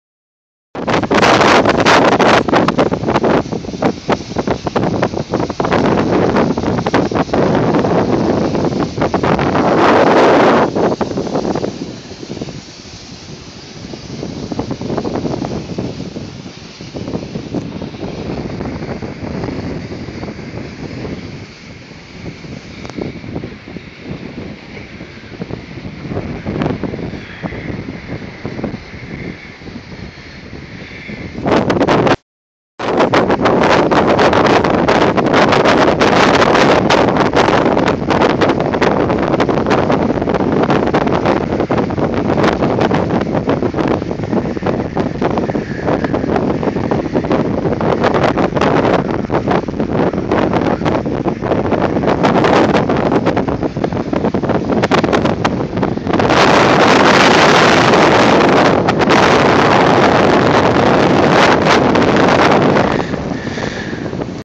Desde lo alto de San Bartolo.
Vista desde lo alto del monte San Bartolo, en La Cepeda (León). Se puede ver el pantano de Villameca, Villar y el resto de Los Barrios de Nistoso.